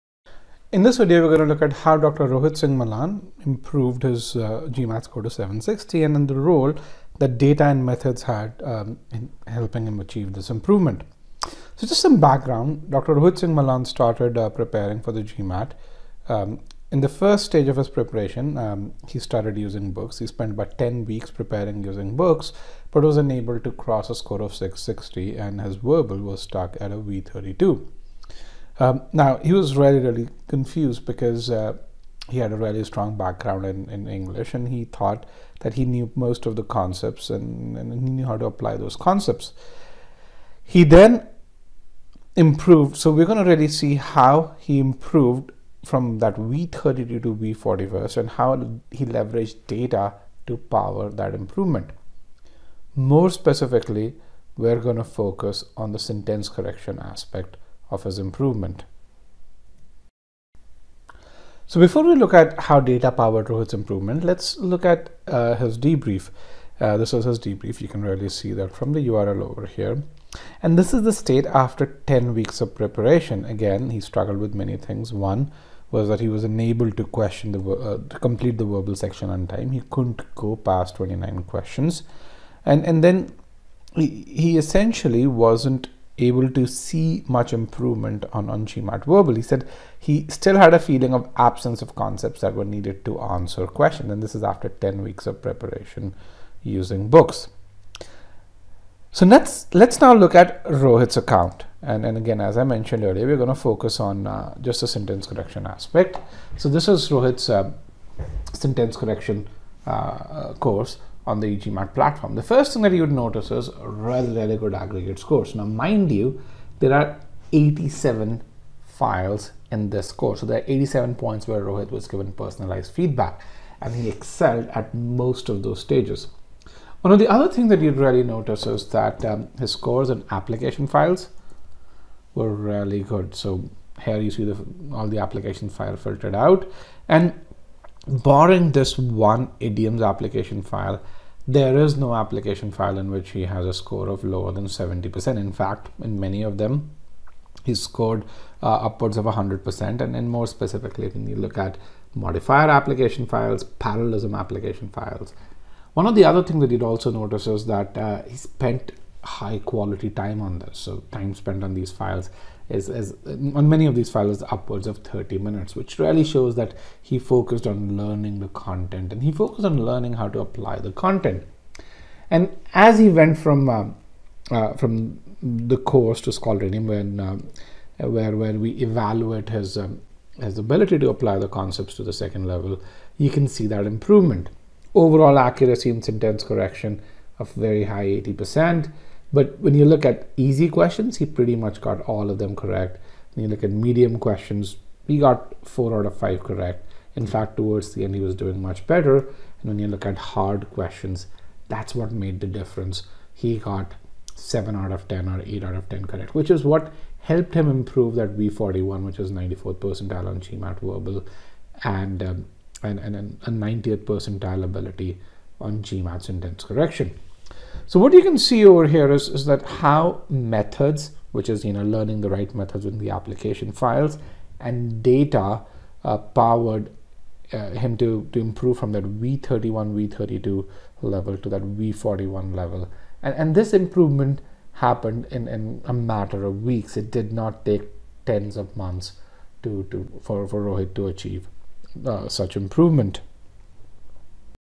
GMAT 660 to 760: How data and methods helped Dr. Rohit improve his GMAT score
Watch the video to know how Dr. Rohit Singh Malan improved his GMAT score from 660 to 760 using data and methods. Dr. Rohit was stuck at a GMAT Verbal score of V32. This video talks about how Dr. Rohit leveraged data to go from V32 (60th percentile) to V41 (94th percentile), and his complete journey powered by data and methods.